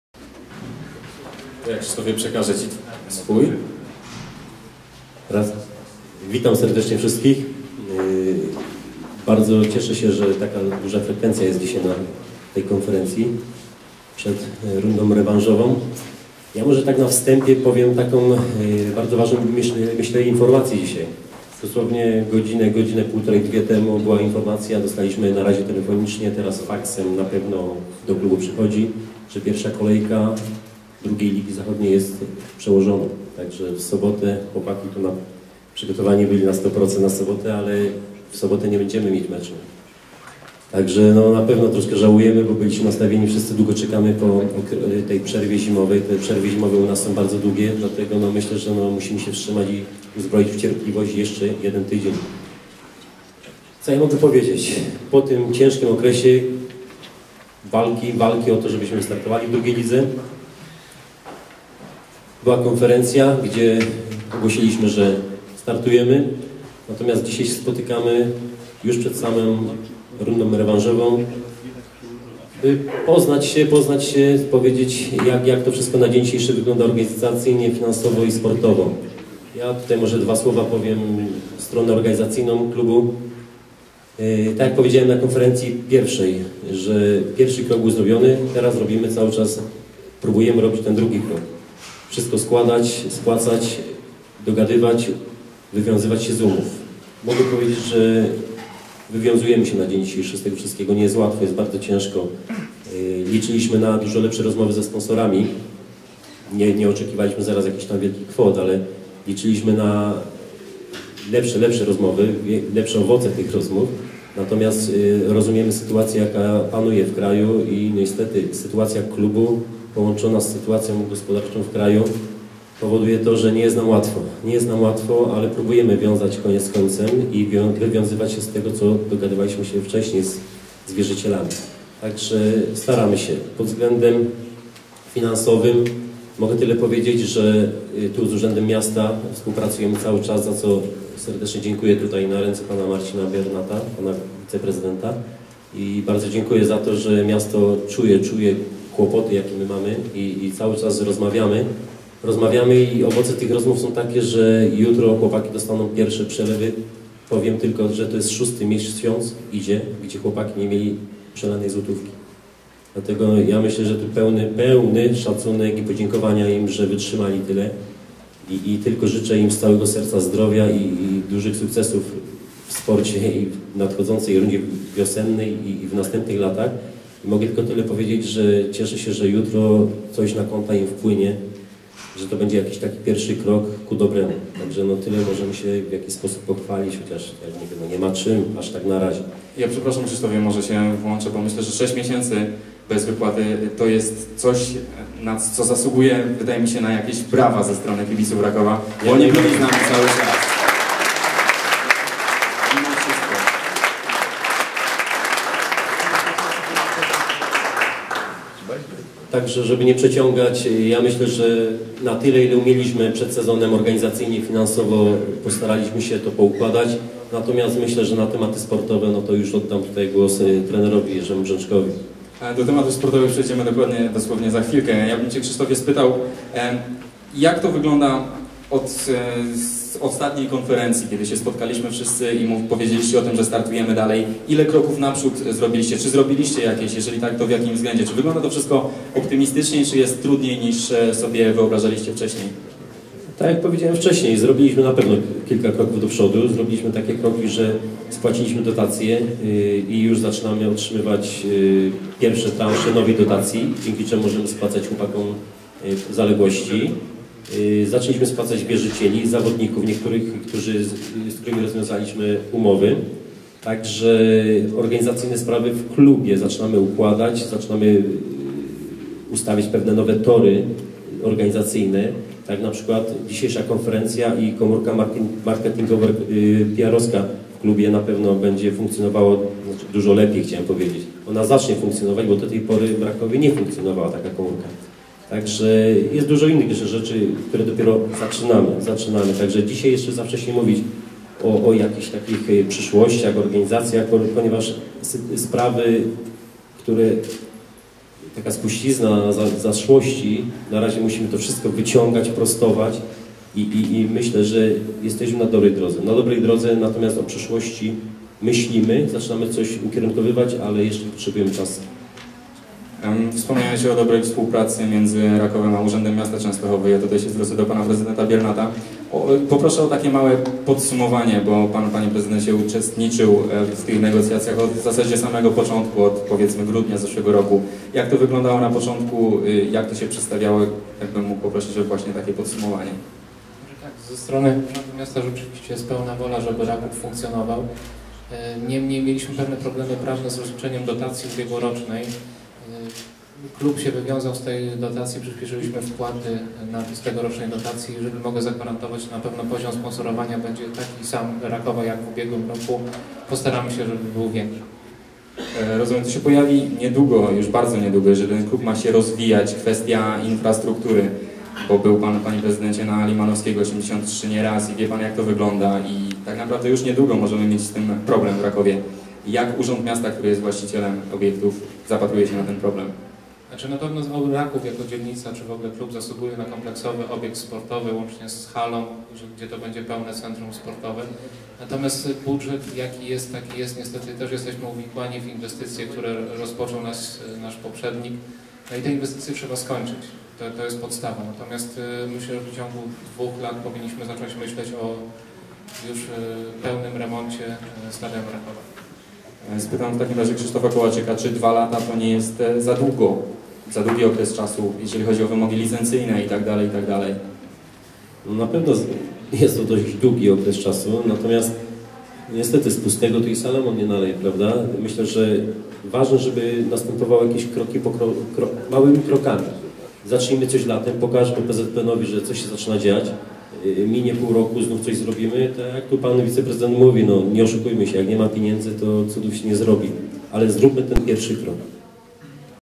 Wracamy do gry! Konferencja - część 2/5
Konferencja prasowa połączona z prezentacją drużyny Rakowa przed rundą wiosenną 2010.